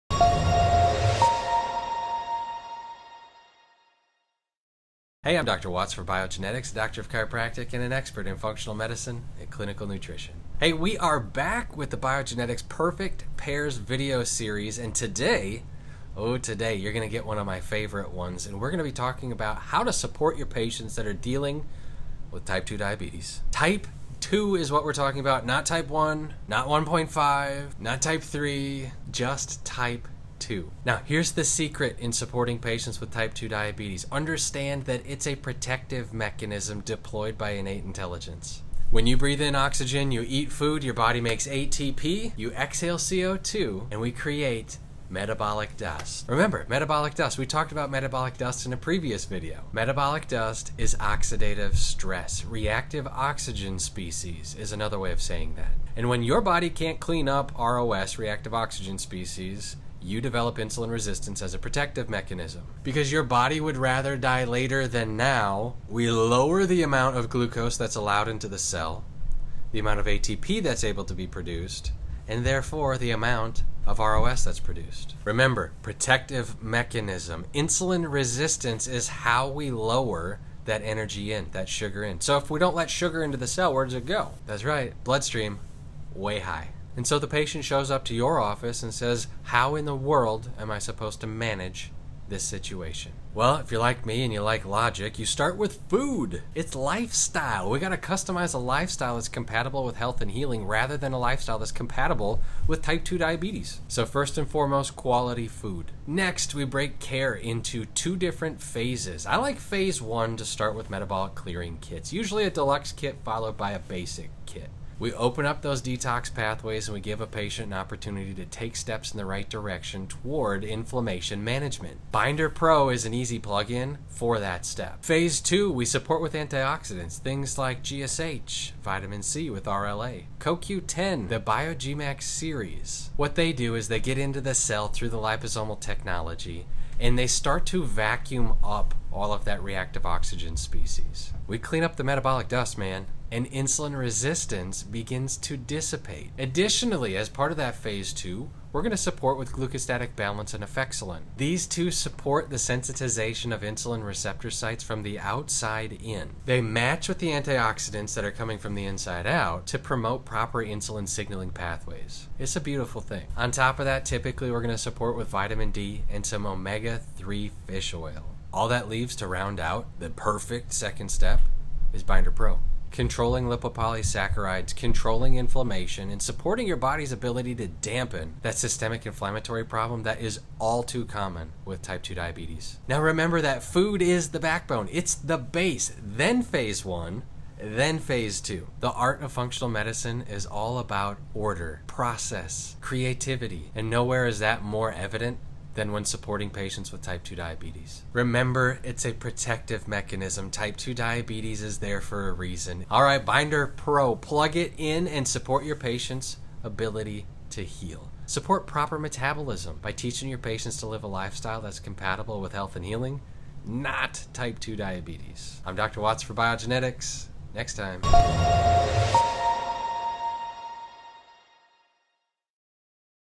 Perfect Pairs Series: Type 2 Diabetes
The Biogenetix Perfect Pairs Series continues with a focus on Type 2 Diabetes and the impact using Binder Pro can have on existing patient protocols.